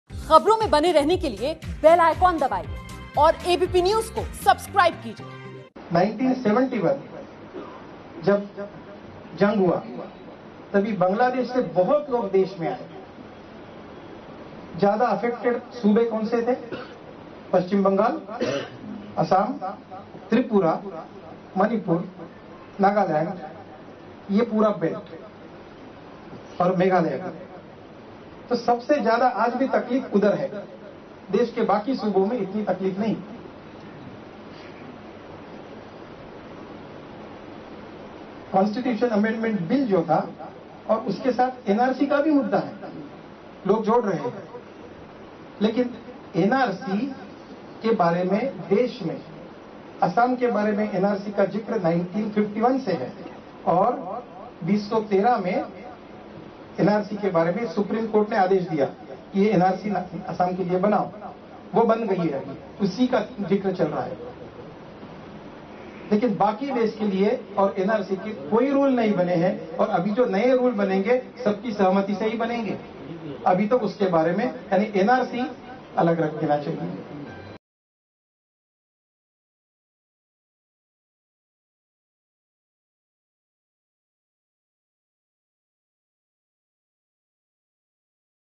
Mumbai: Commissioner Of Police Holds Talks With Members Of Muslim Community Over CAA | ABP News
Mumbai Commissioner of Police Sanjay Barve on Wednesday said that he held talks with members of the Muslim community to clear their doubts regarding the Citizenship Amendment Act (CAA) 2019."Today I had a talk with representatives of Muslim society. It was seen that there were many doubts in the Muslim and Hindu communities regarding the CAA. This is the reason why we held talks with them. We cleared the fact that Indian citizens have nothing to fear about the CAA," said Barve.